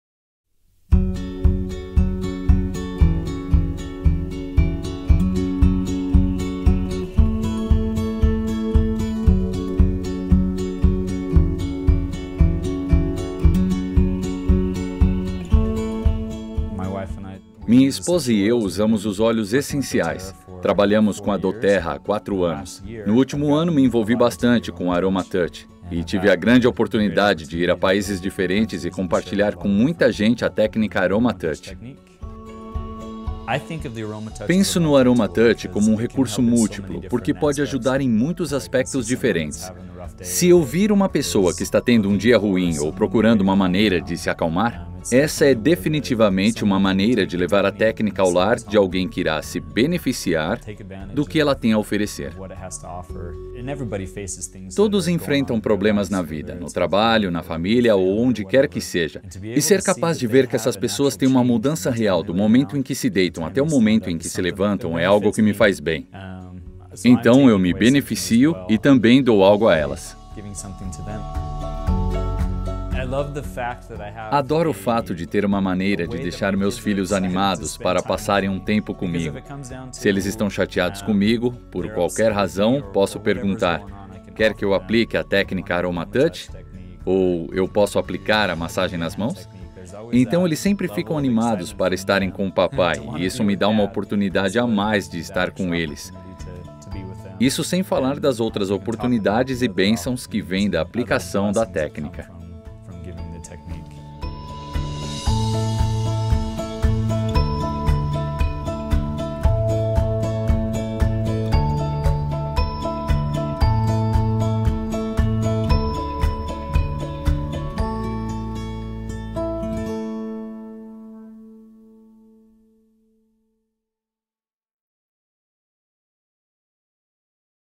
Técnica Aromatouch - Depoimento Andy
A técnica dōTERRA AromaTouch™ é uma maneira simples, porém poderosa, de proporcionar
a cada indivíduo uma experiência renovadora com óleos essenciais.